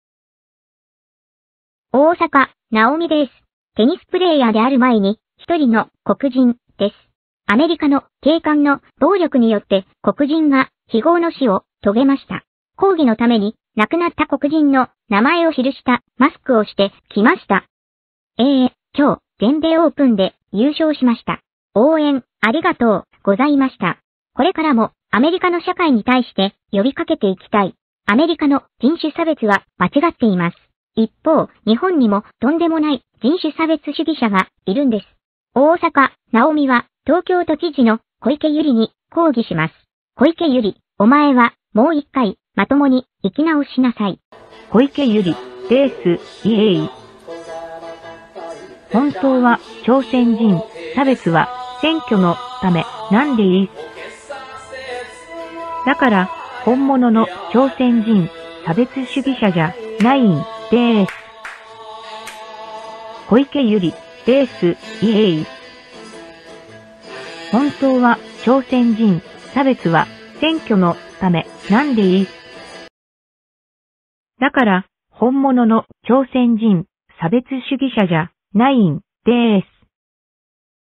大坂なおみです。人種差別に闘います。東京知事の小池百合にも強い反省を求めます
20/9/12　大坂なおみは、全米オープンで優勝をとげた。
黒人差別に対する抗議を続けます。
日本でも、東京知事の小池百合の朝鮮人差別が問題になっています。
小池百合、もう一回、まともに生きてみなさい。いいね